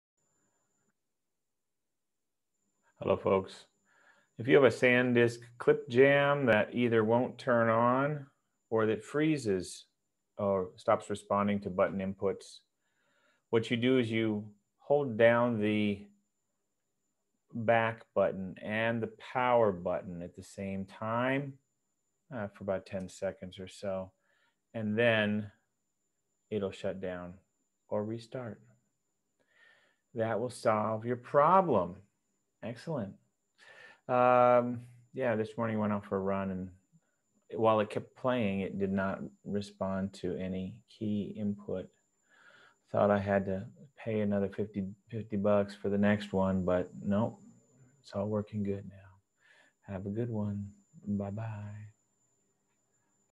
SanDisk Clip Jam -- How to unfreeze -- reset when not responding
The SanDisk Clip Jam and SanDisk Sansa have a reset feature if you hold down the Back + Power button for 10 seconds or so. This will reset if the ClipJam (or Sansa) does not turn on, or does not respond to button input.